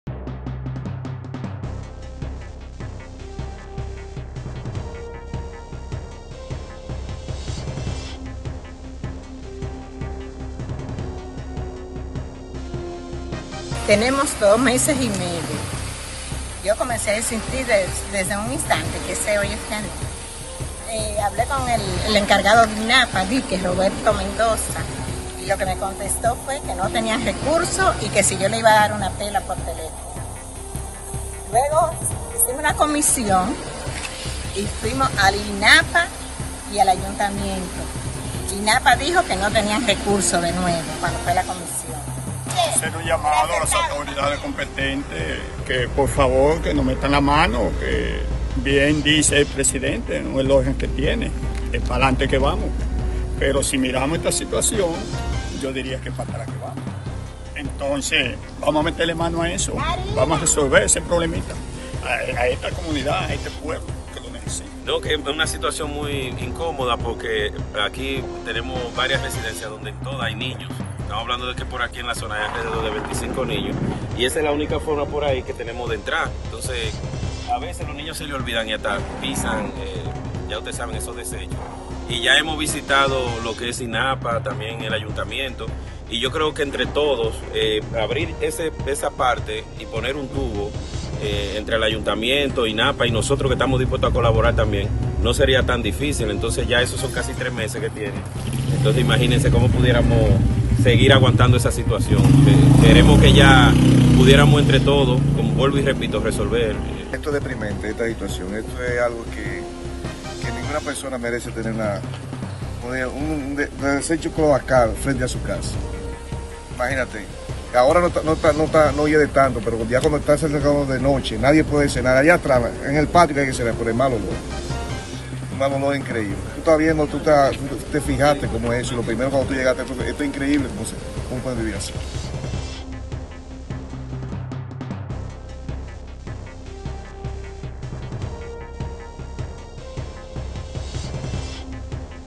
Moradores denuncian cúmulo de aguas residuales en sector El Hospital en Castillo
Duarte - A pesar de las múltiples denuncias de atención a las autoridades, moradores del sector El Hospital en el municipio de Castillo, expresan que a la fecha no reciben respuesta ante el cúmulo de aguas residuales.

El hedor que generan las aguas atenta contra la salud de todos los residentes pues hasta las heces fecales tienen como último destino la parte frontal de sus viviendas.

Basilia Mora afirma que en varias ocasiones han acudido a INAPA y los encargados dicen que no hay presupuesto para resolver la situación.

Agregan, que han tenido que acudir a los medios de comunicación para buscar una salida ya que el hedor es permanente y ni siquiera pueden almorzar con tranquilidad.

Esperan que INAPA y la alcaldía del municipio ofrezcan una respuesta rápida al problema.